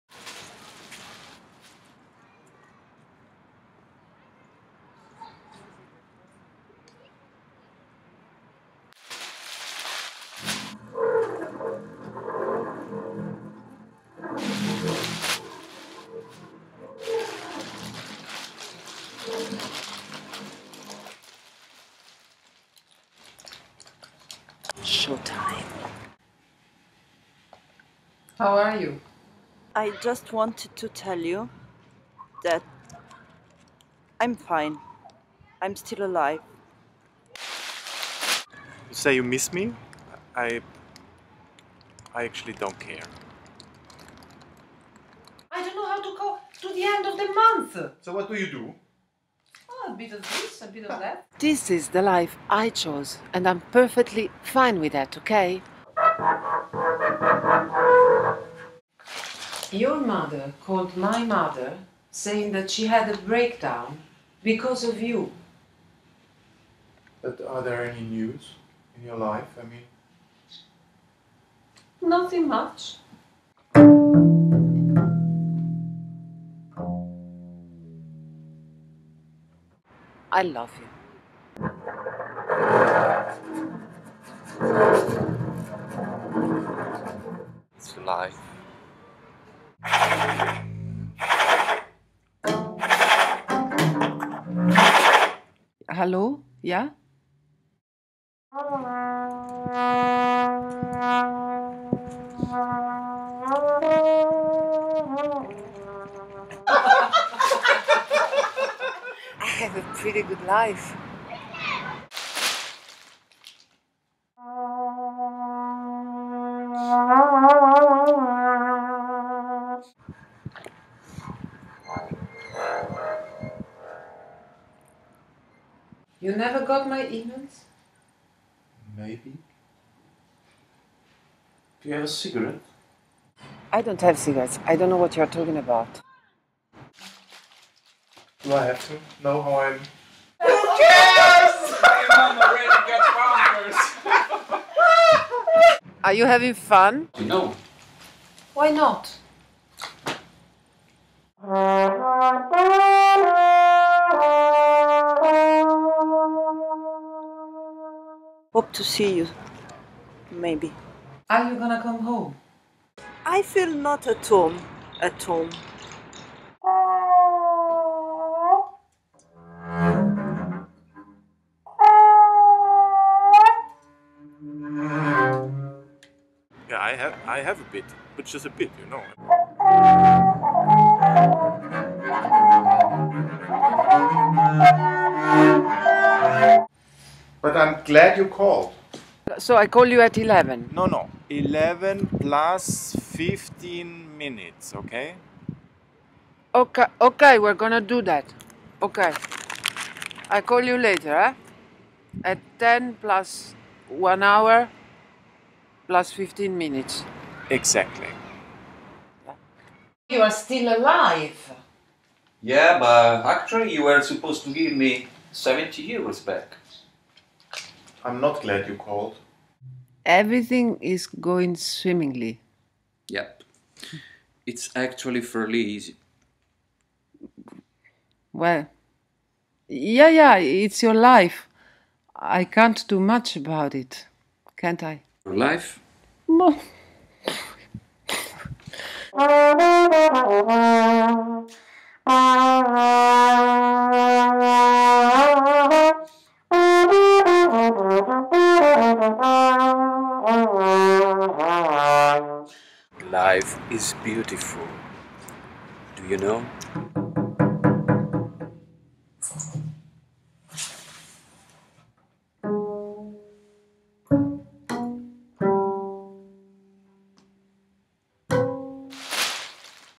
The Grey Area. trailer
AUT & OTTO in a new performance

Sara Ercoli: concept, direction and act
Manuela Giovagnetti: choreography, dance and act
Meinrad Kneer: double bass and act
Davide Piersanti: trombone and act

costumes: laziapazza
light design: Marc Carrera

thanks to Tatwerk Berlin